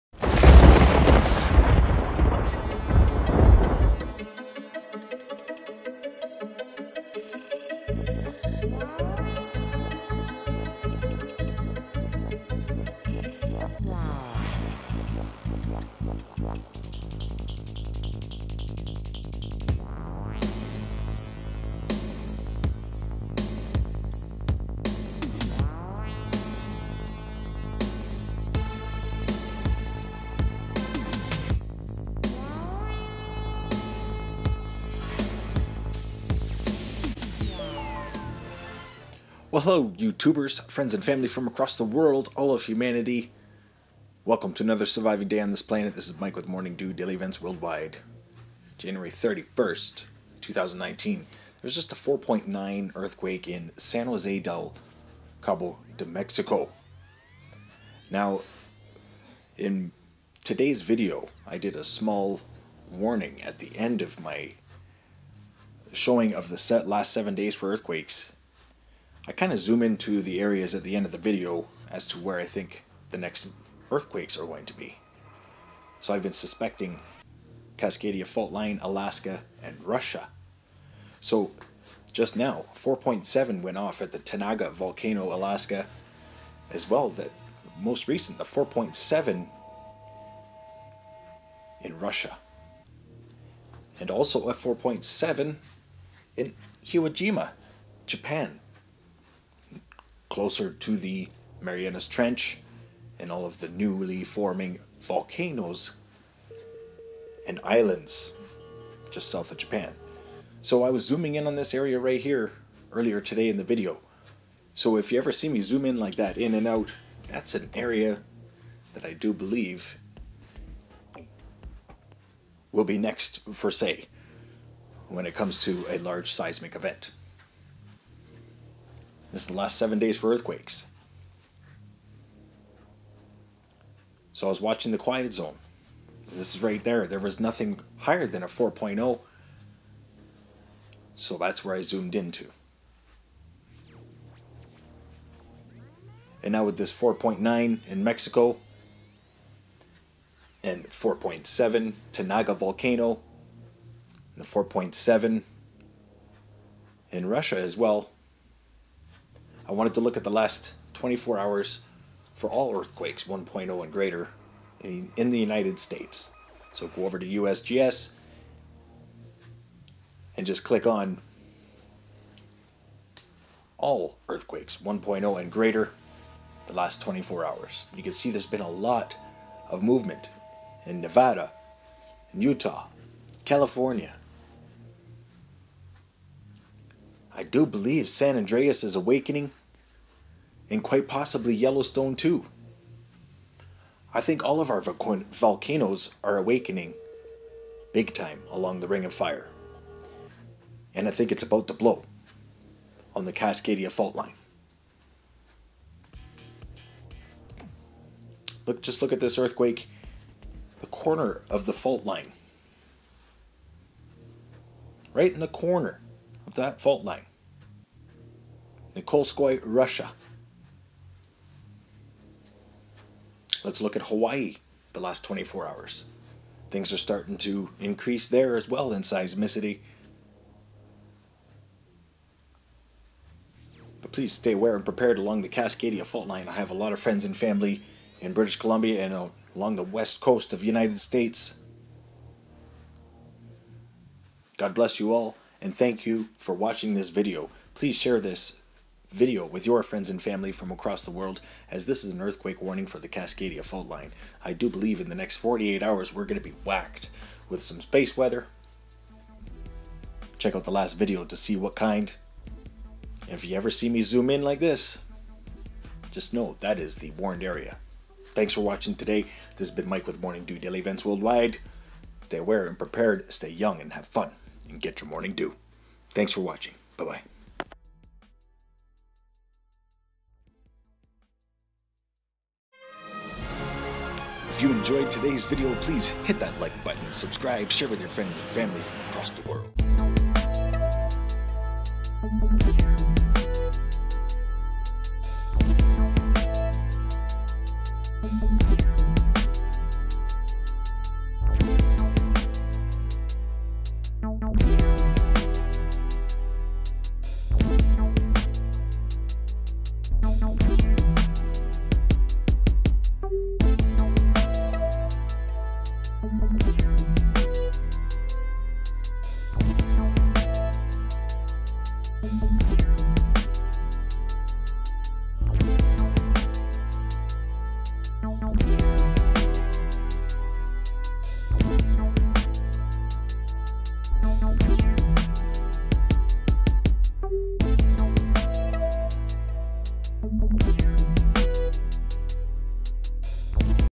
//ALERT\\ 4.9 Earthquake San Jose del Cabo, Mexico / Tanaga Volcano / 4.7 EQ Nikol'skoye, Russia
-                                                Morning D.E.W.

Get Your Winter Gloves, Weather Stations, Basic Camoflauge SURVIVAL packs And other things... Drones, cameras etc.